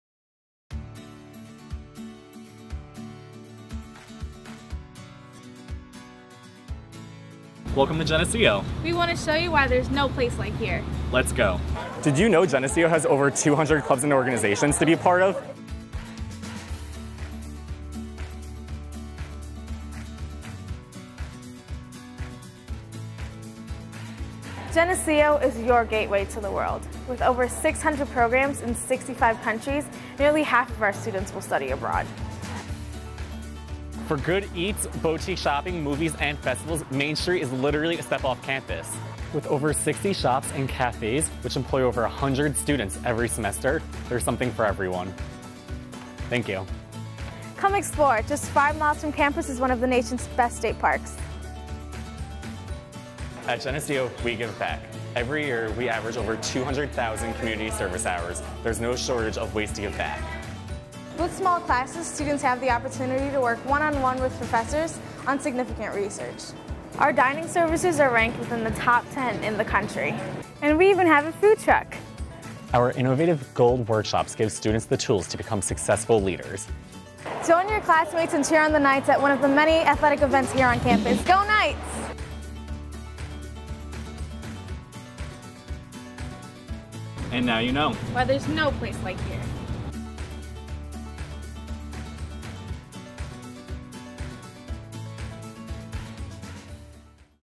Find out why there's no place like here!
Take a tour with Troy Hallahan '19 and Theresa Roberts '18 and find out why there's no place like Geneseo!